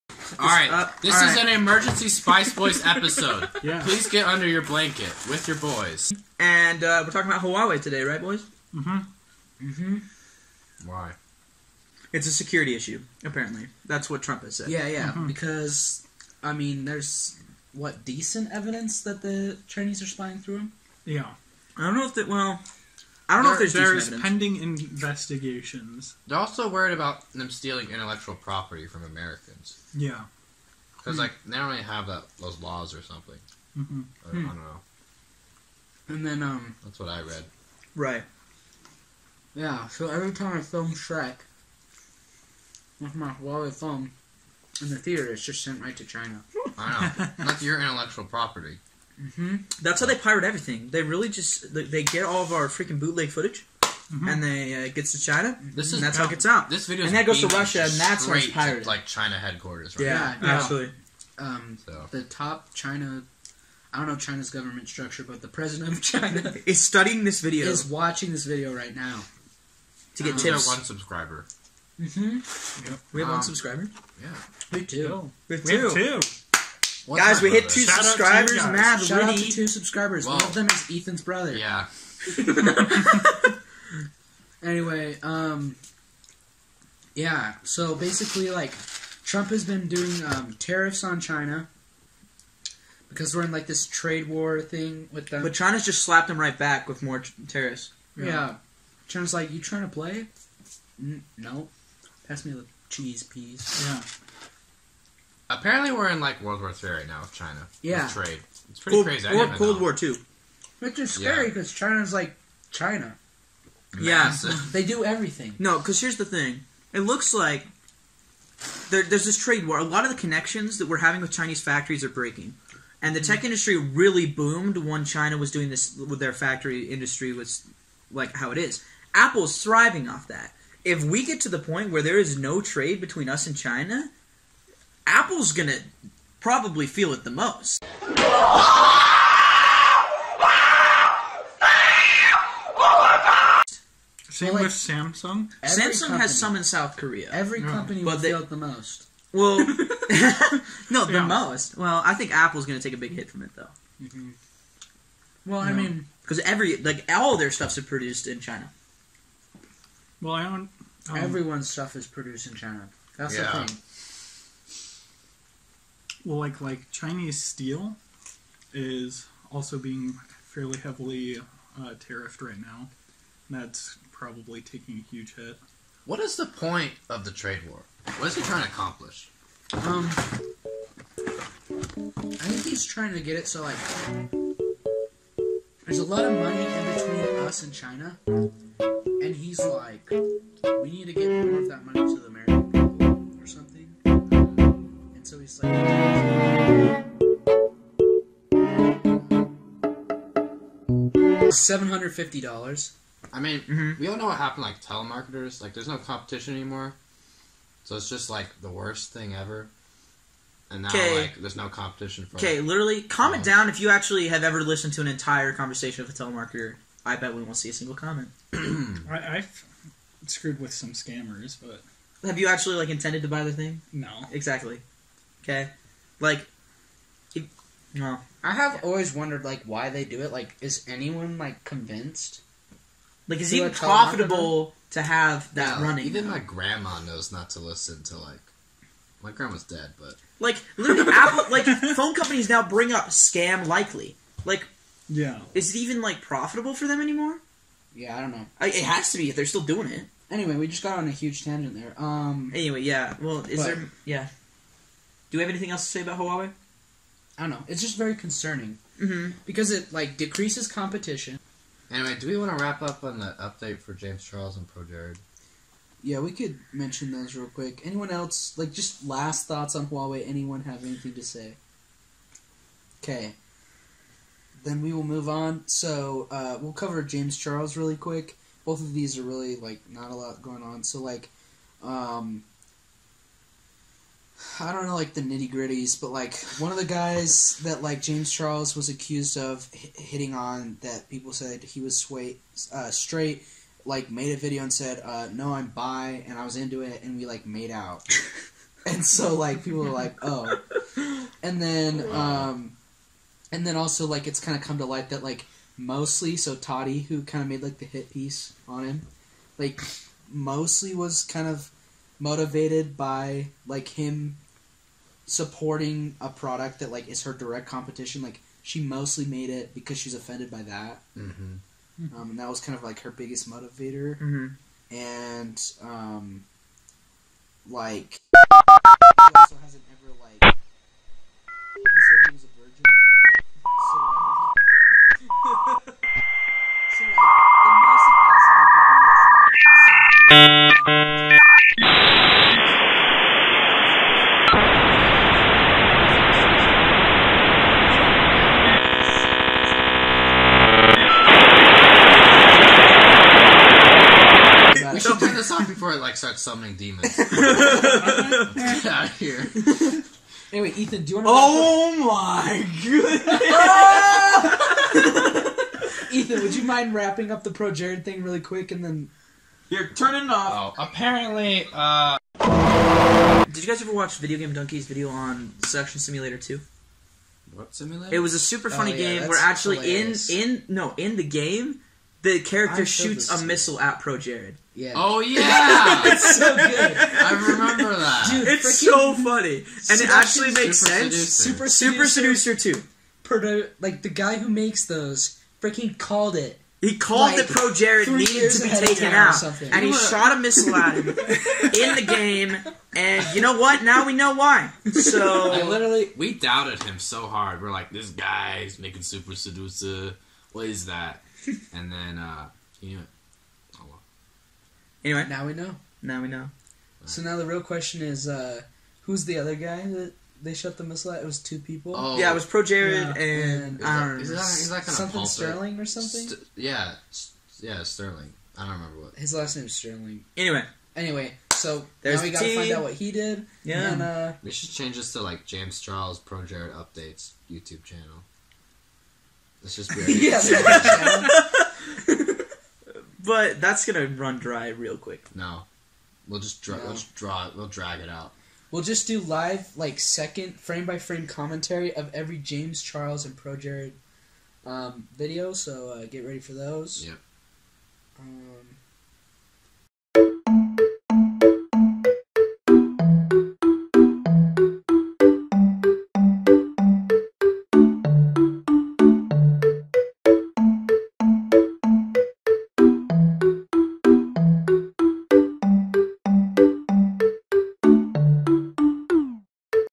Huawei Ban And Other Deadly Sins - Blanket Boys
We discuss the Huawei ban and also update our loyal fan base on James Charles and Projared.

Remember to use code SPICE to get one month free of dollar shave club.